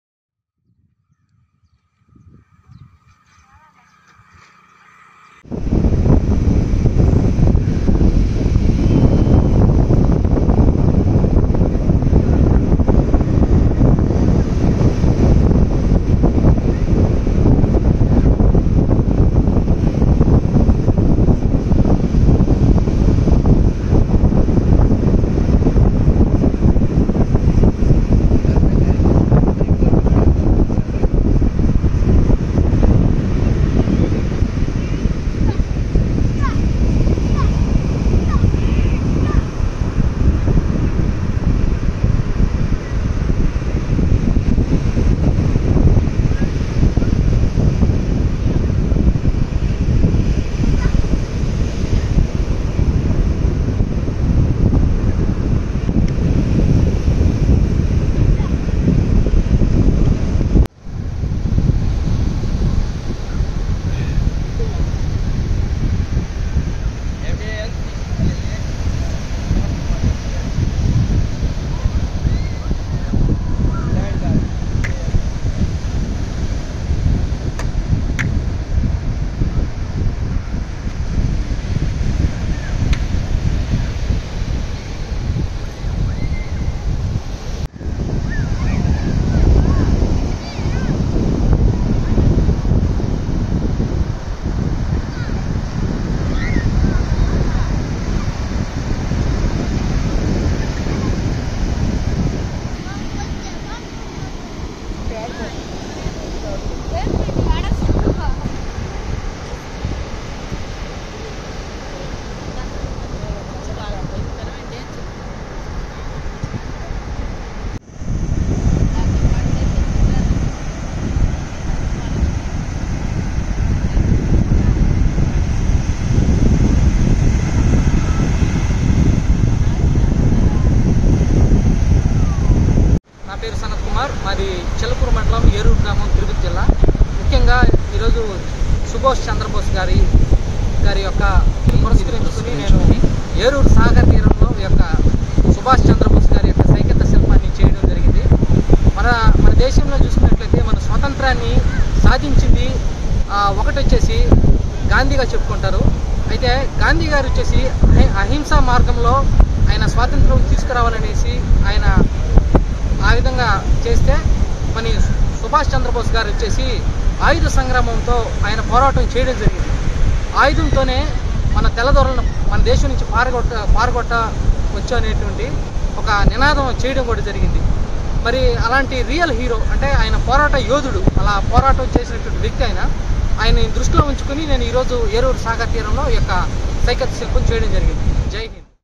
సాయుధ సంగ్రామమే న్యాయమని.. గగన సిగలకెగిసి కనుమరుగై పోయాడు.. - NDN News
సాయుధ సంగ్రామమే న్యాయమని..
గగన సిగలకెగిసి కనుమరుగై పోయాడు..
సాయుధ సంగ్రామమే న్యాయమని.. గగన సిగలకెగిసిన నేతాజీ పుట్టినరోజును పురస్కరించుకుని సైకతశిల్పాన్ని రూపొందించారు. చిల్లకూరు మండలం, ఏరూరు గ్రామానికి చెందిన సనత్ కుమార్ ఈ సైకత శిల్పాన్ని తీర్చి దిద్దారు. స్వాతంత్య్ర పోరాటంలో నేతాజీ పాత్ర మరువలేనిదని చెప్పారు.